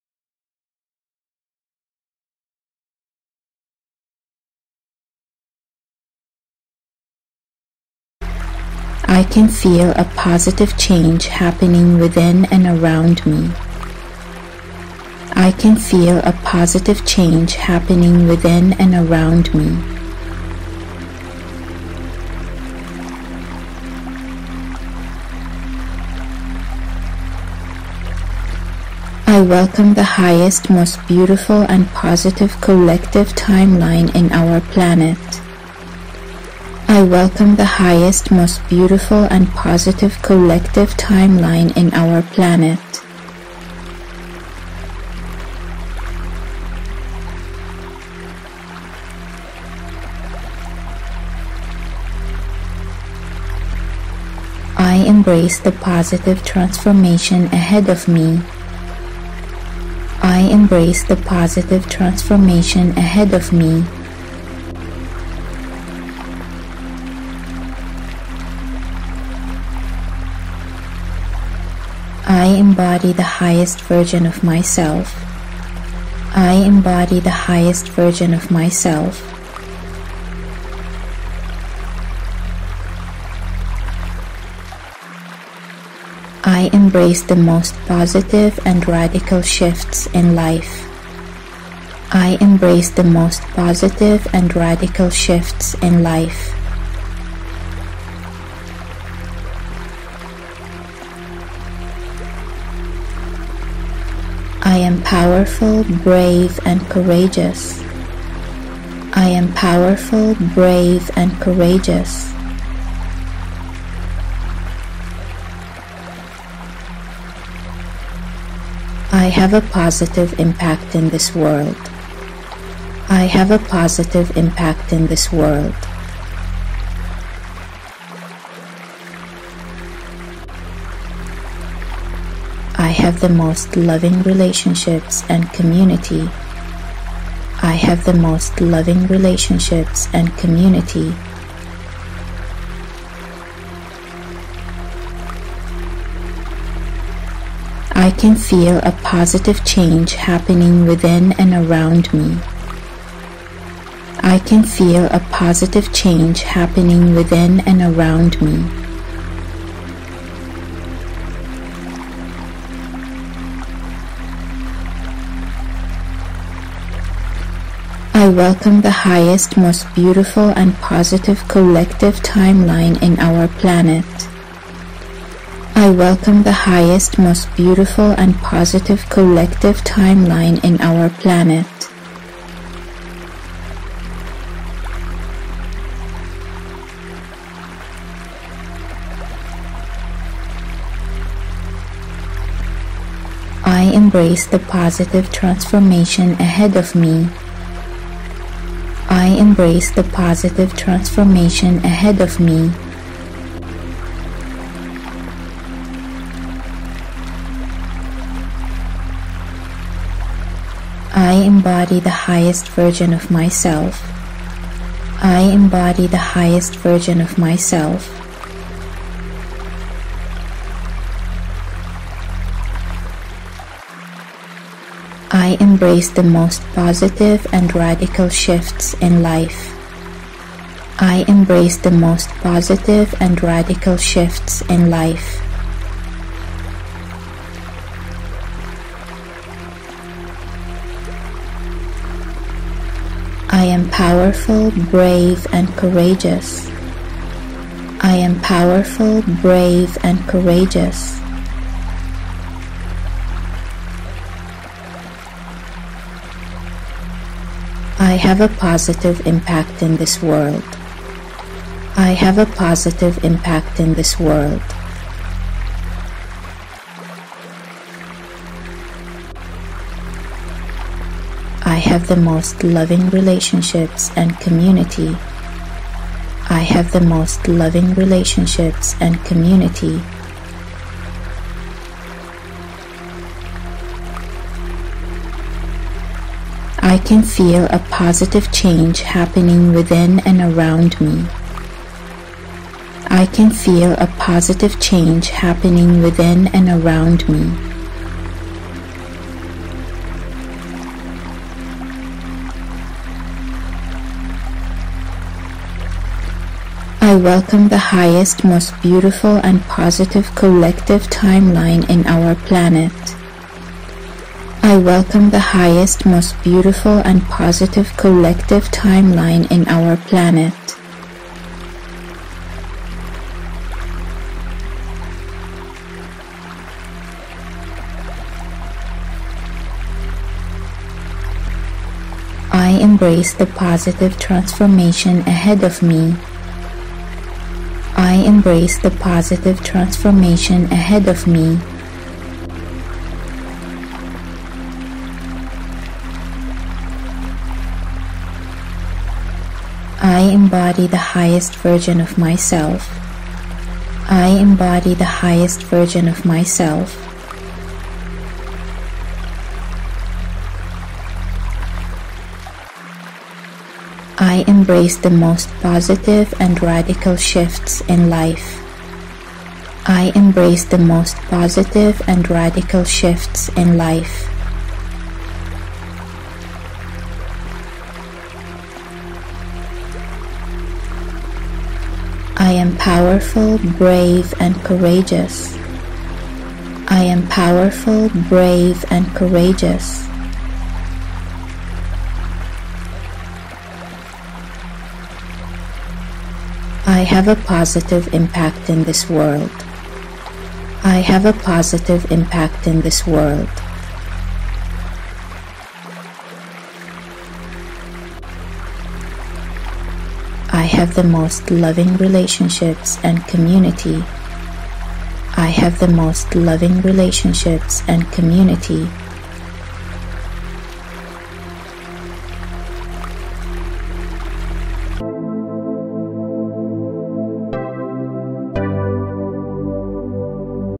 Pluto in Aquarius | affirmations
as we welcome in Pluto in Aquarius energy in 2024, a huge transit that will last for at least 20 years, I invite you to use these affirmations to support your journey and align to your highest timelines. These affirmations are powerful declarations and healing supportive tools on your journey ahead.

Repeat after me loudly or in your mind.

Thank you for all of your love and support.